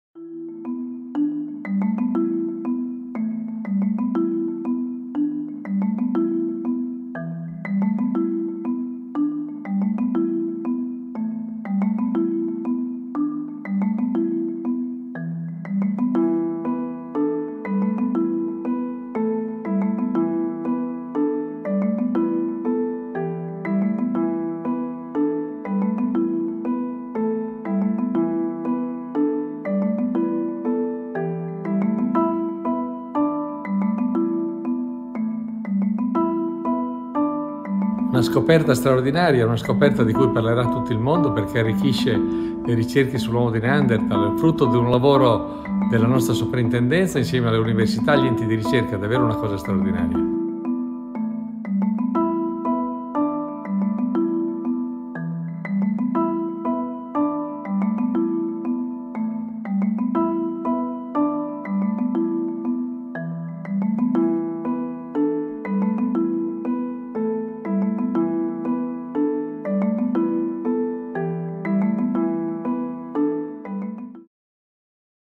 'Exceptional' discovery of Neanderthals in Italian cave: minister | AFP
The fossilised remains of nine Neanderthal men have been found in a cave in Italy, with the discovery described by Italy's culture ministre as 'truly exceptional'.